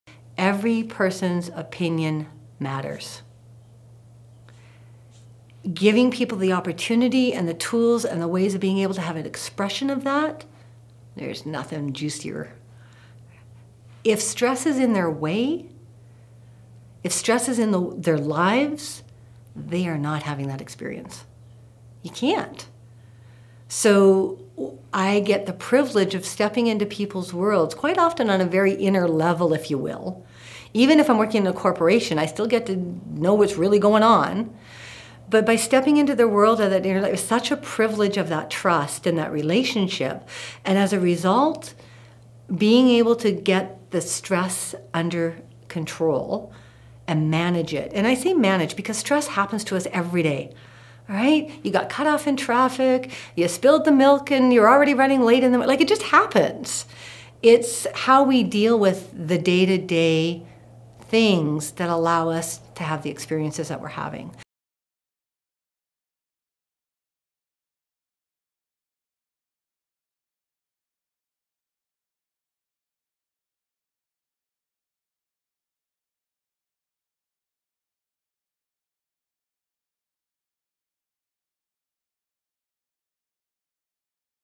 Janice Otremba - In the Workplace... Every Person's Opinion Matters
Every person's opinion matters. Although some individuals have a tough time expressing their opinions, Janice Otremba is able to help individuals express their opinion in a professional way. Janice teachers her clients appropriate measures to deal with daily stress. By learning to manage your stress, you are able to live in the moment and experience the true essence of life.